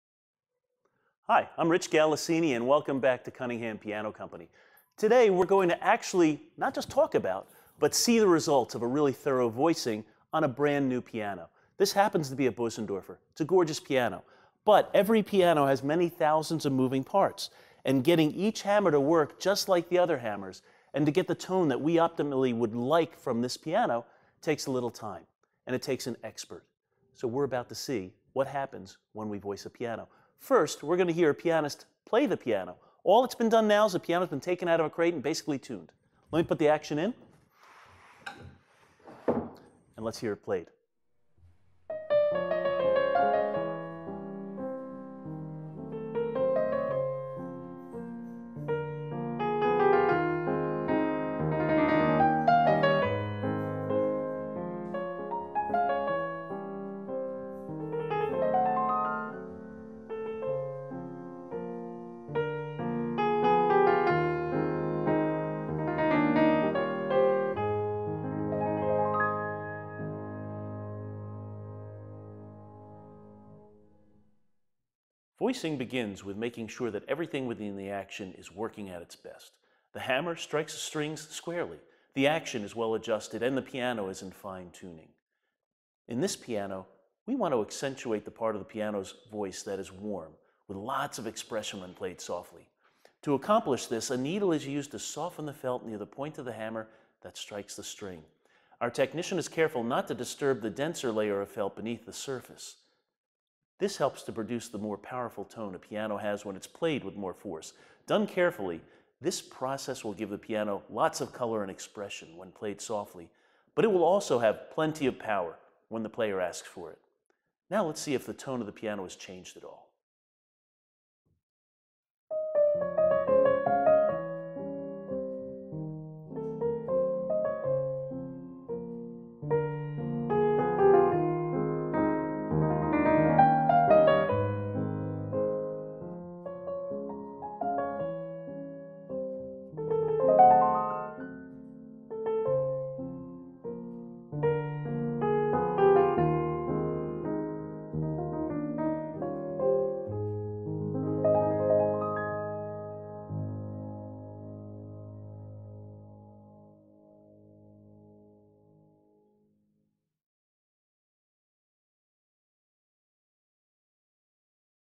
Voicing a Piano | Cunningham Piano Company
Piano voicing is the process of adjusting the density of the felts covering the hammers striking the string to produce a warmer or brighter tone. In this video, we will see how a piano is voiced and hear what a difference it can make to the piano’s overall sound. For more information on Cunningham Piano Company, visit our